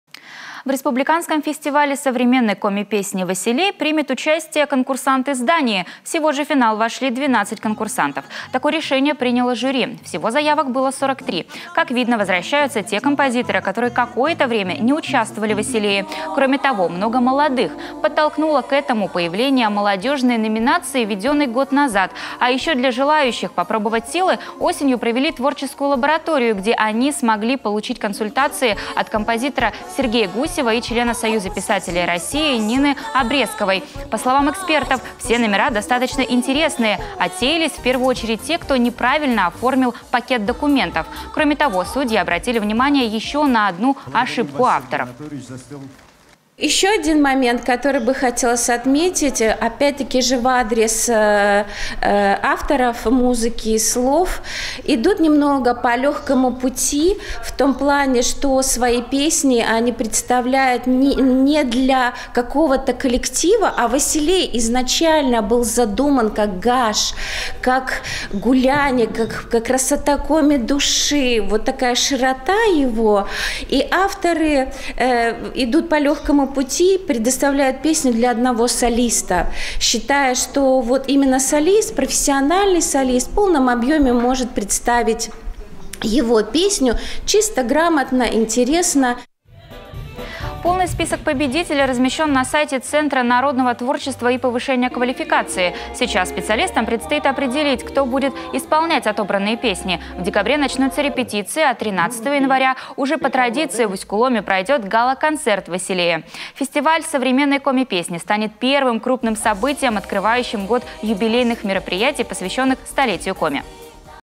В Республиканском фестивале современной коми песни «Василей» примет участие конкурсант из Дании
В Республиканском фестивале современной коми песни «Василей» примет участие конкурсант из Дании, а всего в финал вошли 12 конкурсантов. Такое решение приняло жюри. Всего заявок было 43. Как видно, возвращаются те композиторы, которые какое-то время не участвовали в «Василее». Кроме того, много молодых. Подтолкнуло к этому введение молодёжной номинации, введённой год назад.